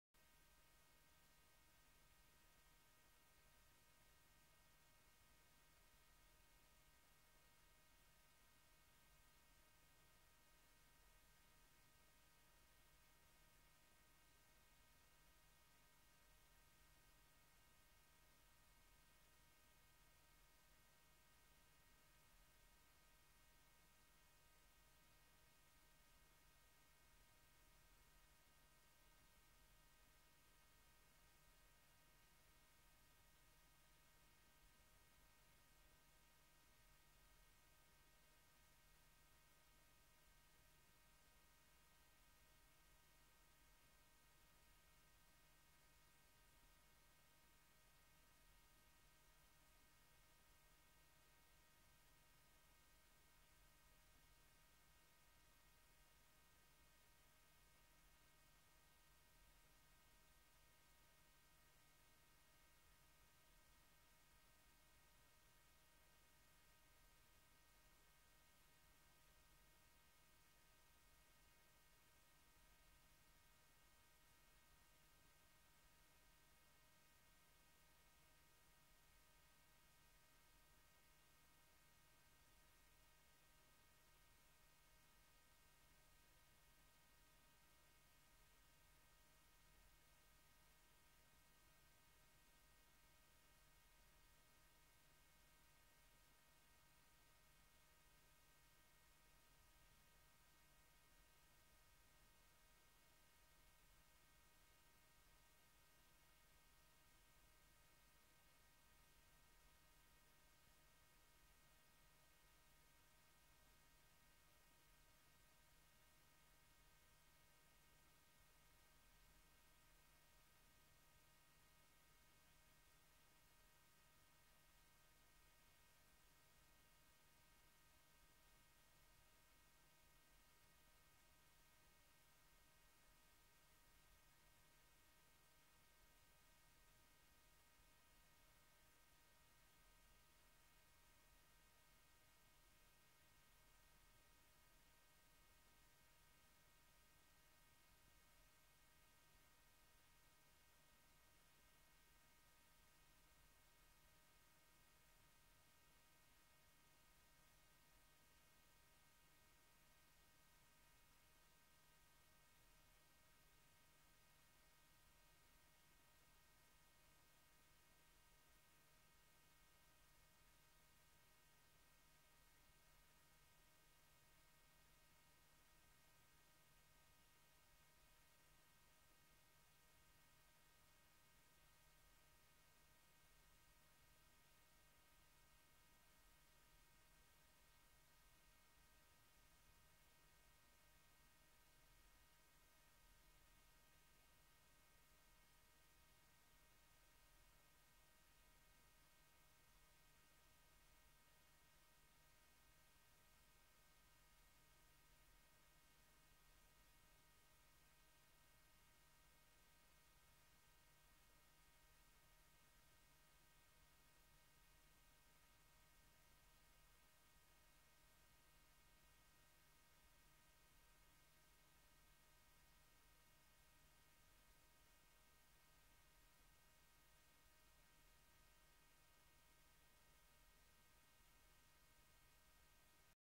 Police rider party (Weeknummer 22-04) 🔴
Description (more): Demonstration mounted police the Hague (founded 1914) on field under much public interest. Turn: saluerend rider Corps; CU paardenkoppen; div. stunts riders (portrait and landscape orientation on horses back 20 "); riders in Ivy (30 ");
Dressage conducted by police officer with whistle, Cap and glove (cu, looking in camera) (21 "); the Mack of the horses with whips by two-by-two trotting riders (34 "). Short between shots, observing public.